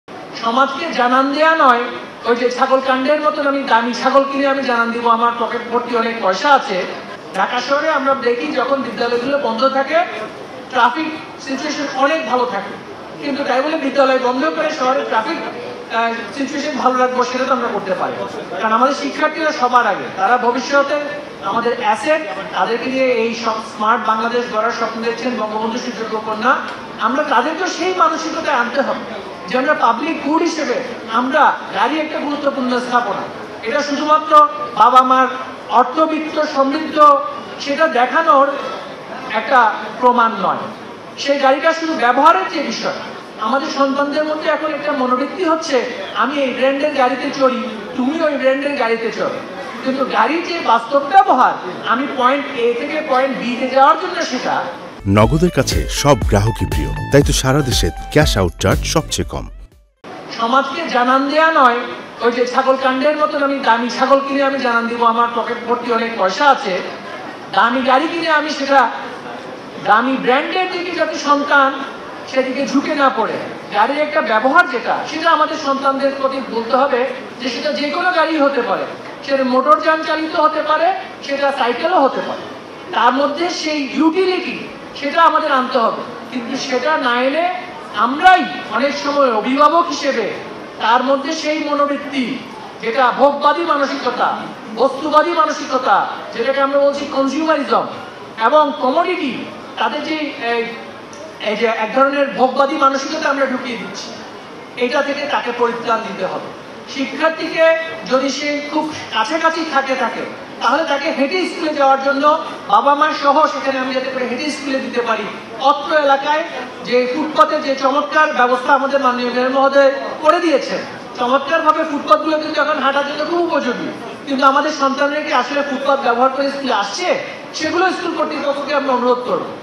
১৫ লাখ টাকার ছাগল কিনে সমাজকে জানান দিবো না আমার পকেট ভর্তি টাকা আছে: শিক্ষামন্ত্রী | Channel 24
১৫ লাখ টাকার এক ছাগল নিয়ে এখন তোলপাড় সামাজিক যোগাযোগ মাধ্যম। এক রাজস্ব কর্মকর্তার ছেলে ঈদে এই ছাগলটি কোরবানি করছেন এমন পোস্টের পর দেশজুড়ে শুরু হয় সমালোচনার ঝড়। যদিও সাদিক এগ্রো বলছে, ১ লাখ টাকা বায়না করেও শেষপর্যন্ত আর ছাগলটি কেনেন নি তিনি।

»» Office Address:
Bangladesh.